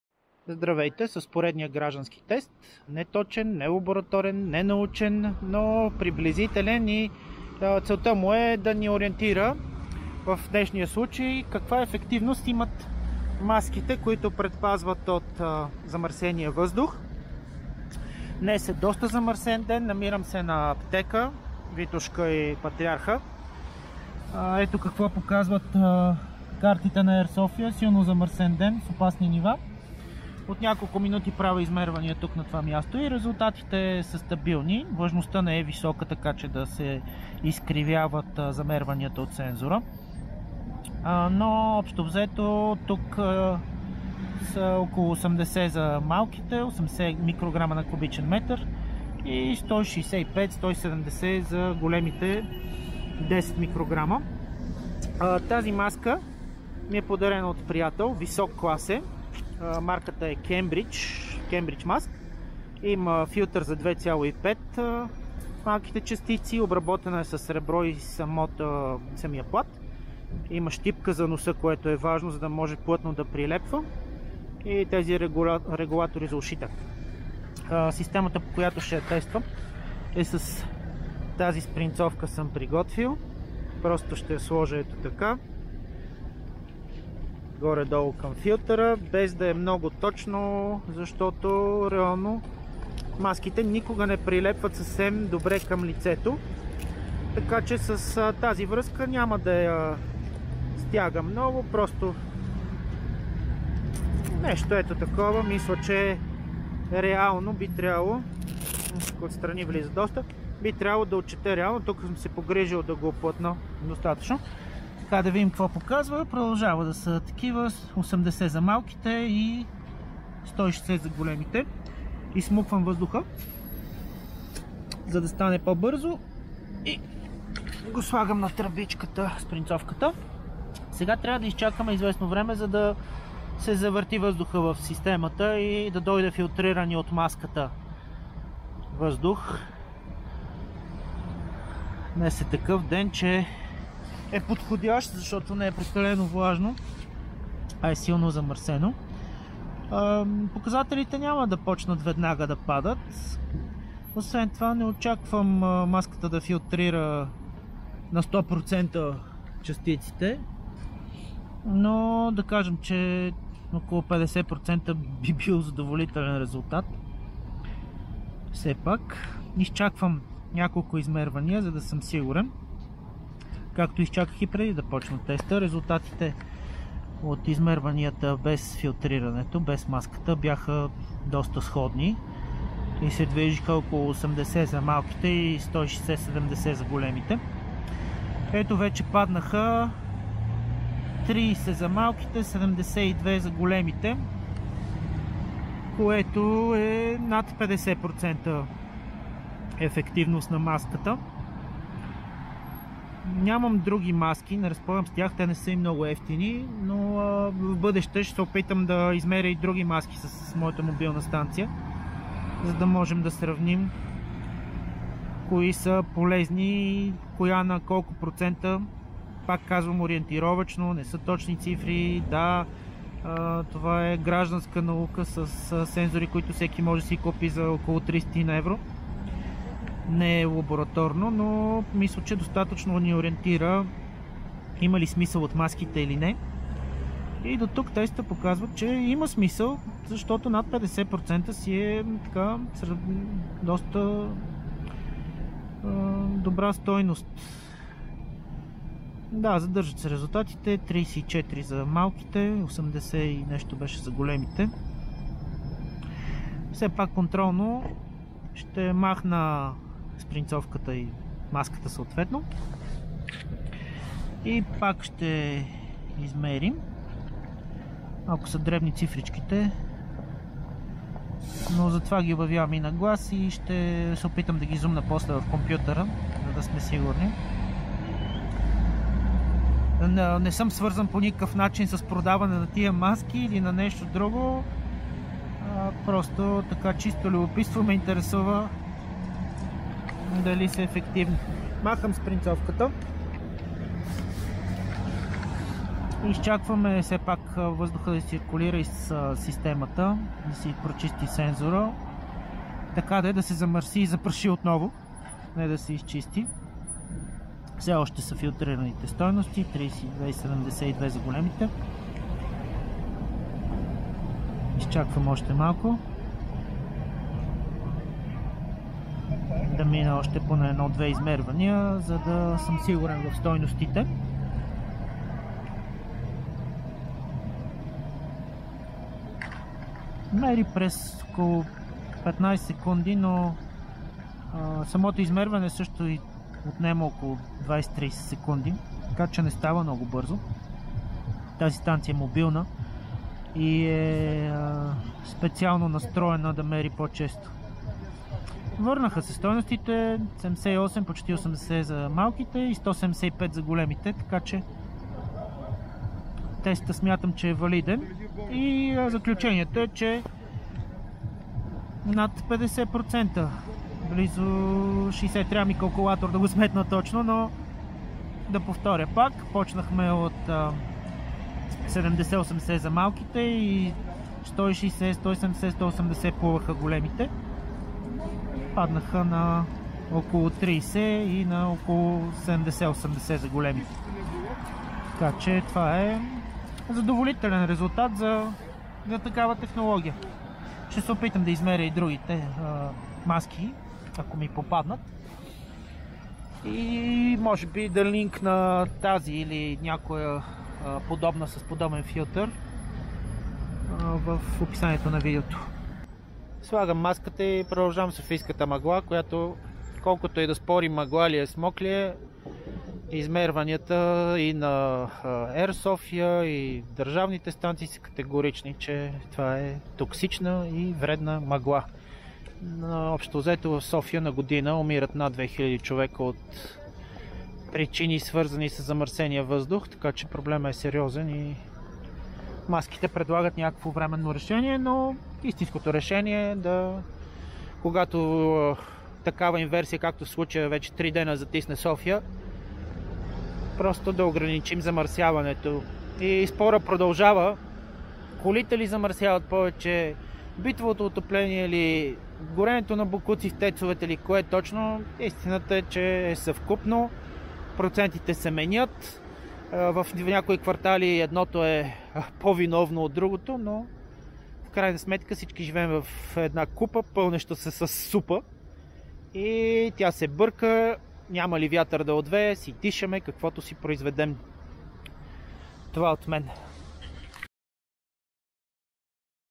Колко предпазват маските за замърсен въздух - ФПЧ
Спира над 50% от ФПЧ 2.5 и 10
има и в амазон

Проведох граждански тест с мобилна станция, като измерих колко е ефективна маска с филтър срещу замърсен въздух.
Проведохме още тестове. Изолирахме само филтъра на българските маски Аirlief, 3M иCambrige. Стойностите, които измерихме са намаляване на вредните частици с над 90%. Оказва се, че материалите и филтрите са добри, по-скоро дизайните и прилепването на маските свалят ефективността им до 50%. Ще разработим още тестове, за да разследваме условностите.
При всички положения: Има смисъл от носенето на маска!